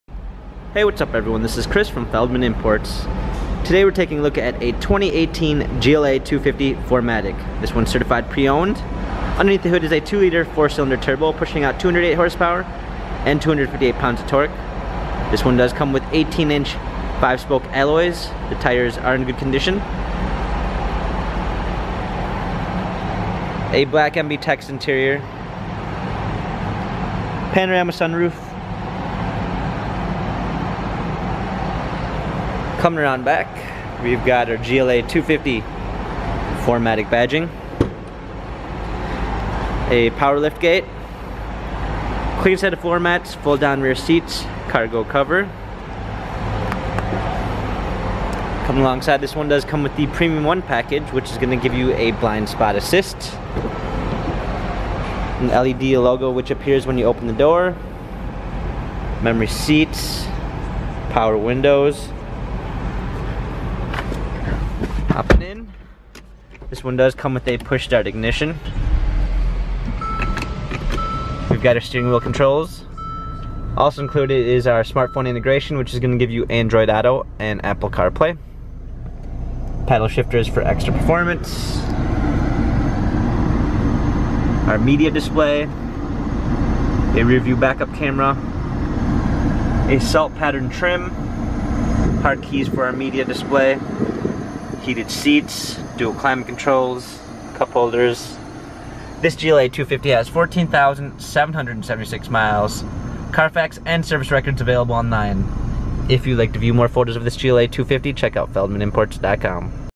2018 Mercedes-Benz GLA 250 4MATIC® - For Sale P13700
Take a look at the 2018 Mercedes-Benz GLA 250 4MATIC® now available at Feldmann Imports. For more information including pricing and availability, please visit us